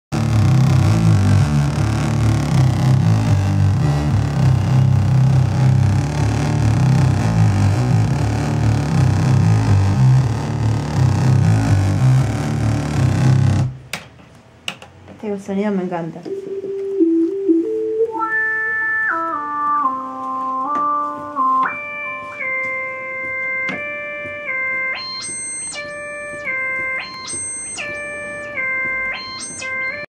Lupi jugando con el Korg MK1
Buscando el sonido perfecto para ohno con el Korg MK1
Looking for the perfect sound with Korg MK1